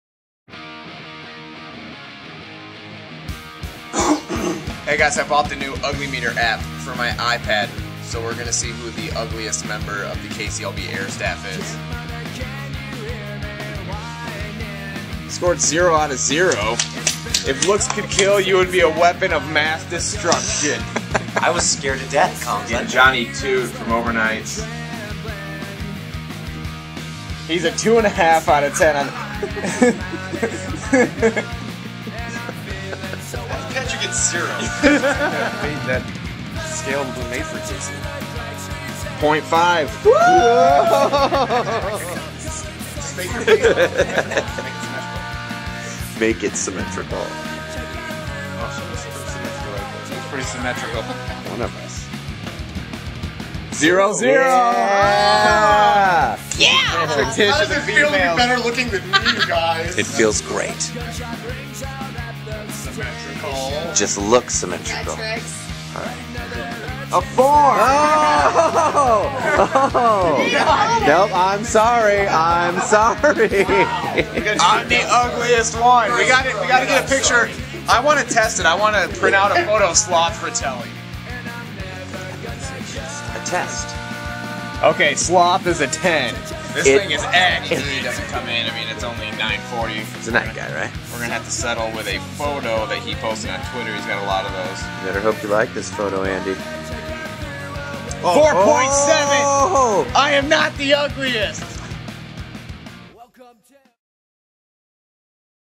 KCLB Backstage - The Ugliest Staffer
Via the Ugly Meter App.. we find out who is the ugliest member of the KCLB Staff @conklinsucks